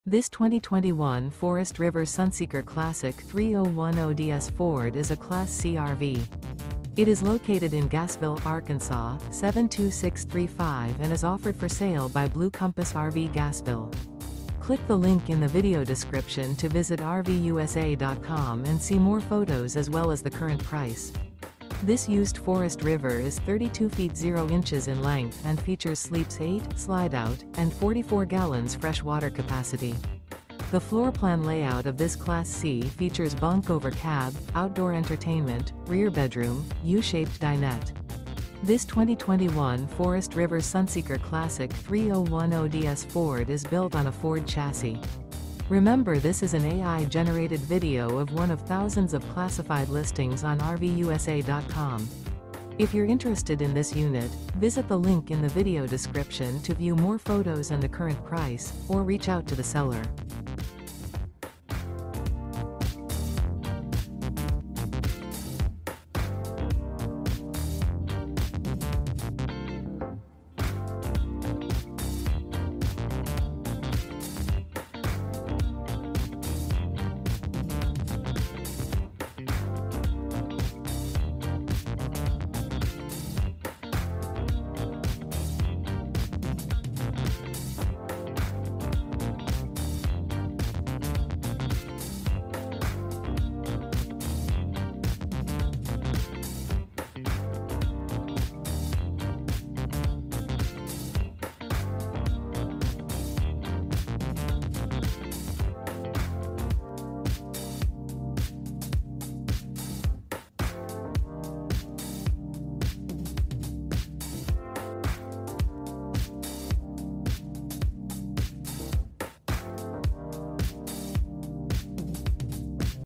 Remarkable 2021 Forest River Sunseeker Class C RV For Sale in Gassville, AR | RVUSA.com
This 2021 Forest River Sunseeker Classic 3010DS Ford is a Class C RV. It is located in Gassville, AR 72635 and is offered for sale by Blue Compass RV Gassville. This Used Forest River Is 32' 0" in length and features sleeps 8, Slideout, and 44 gal fresh water capacity. The floorplan layout of this Class C features Bunk Over Cab, Outdoor Entertainment, Rear Bedroom, U-Shaped Dinette. This 2021 Forest River Sunseeker Classic 3010DS Ford is built on a Ford chassis.